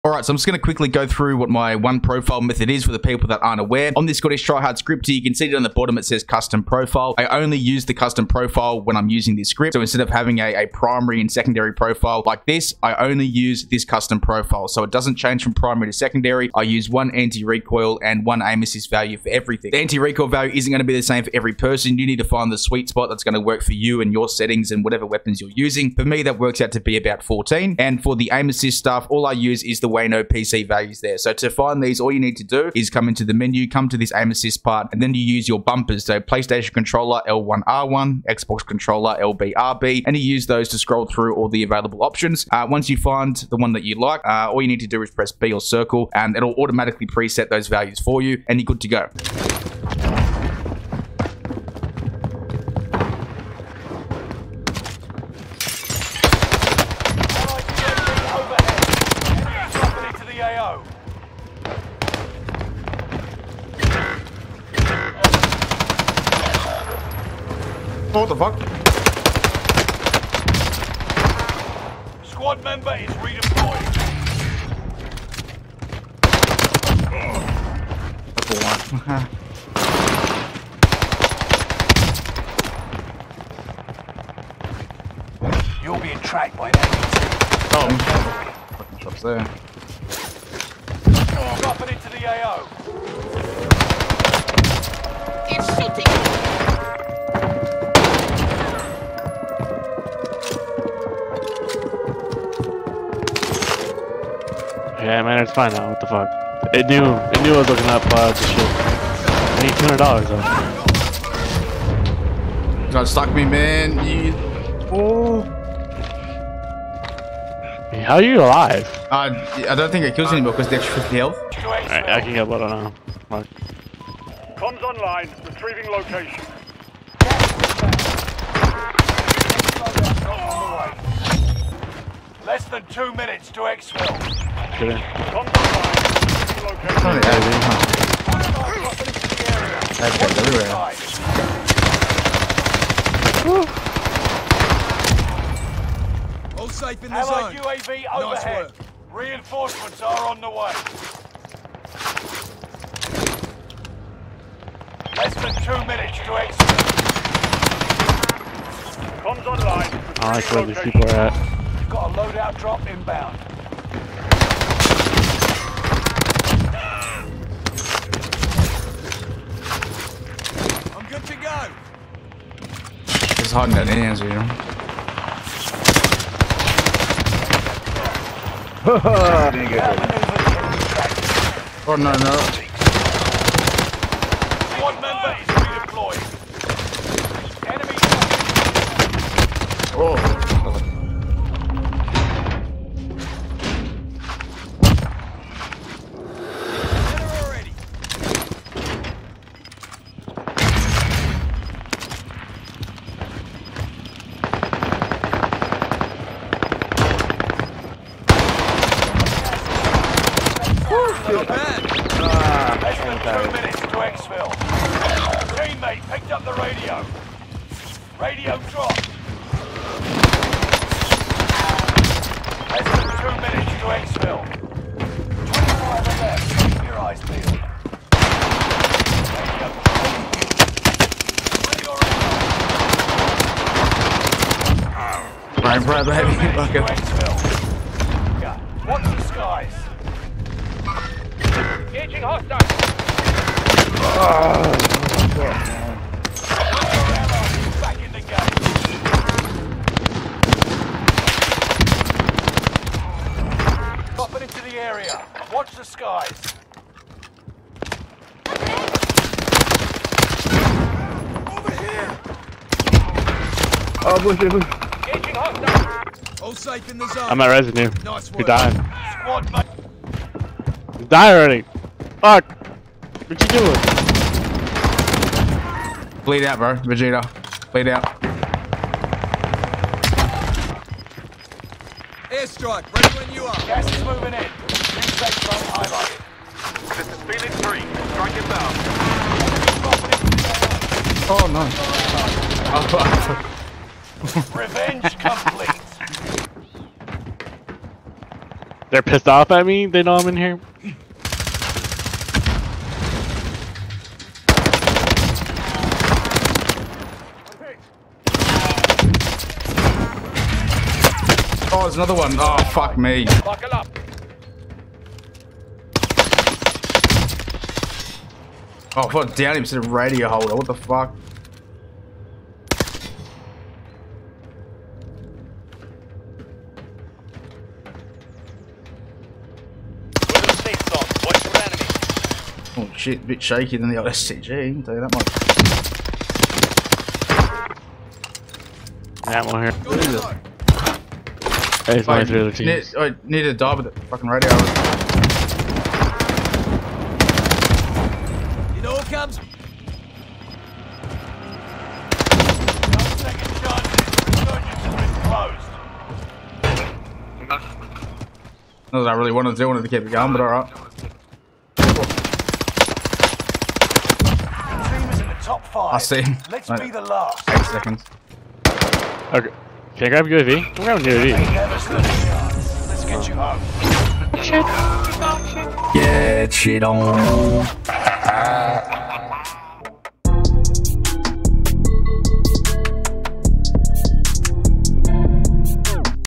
Bots Galore In Exfil Trios | Automaton & Type 100 | Taylordrift Scottish Try Hard 4.1 | Cronus Zen
Tried out exfil trios for something different and got a massive bot lobby. Running my one profile method which I show how to set up It's for an older version of this script but you still set it up the same way.

(After joining the Discord you must click the ''ok'' emoji in the rules channel and then go to the downloads channel to download the script.)